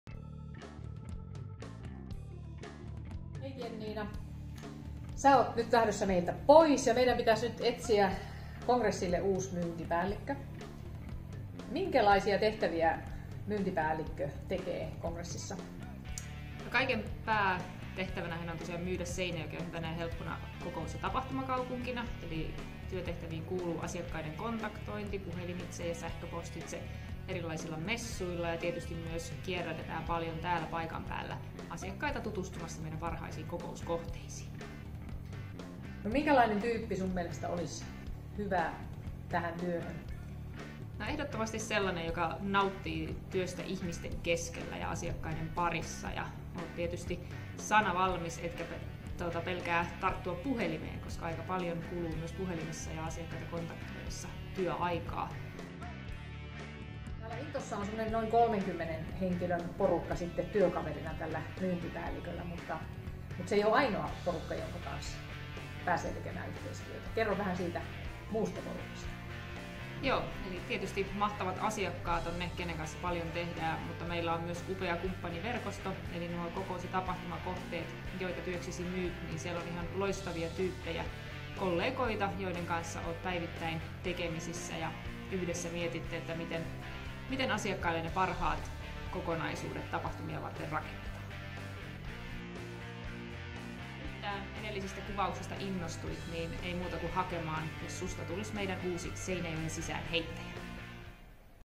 Seinäjoki Congressille haussa uusi myyntipäällikkö
Nykyisen myyntipäällikön siirtyessä uusiin haasteisiin haemme Seinäjoki Congressille uutta  myyntipäällikköä, seuraavaa Seinäjoen sisäänheittäjää.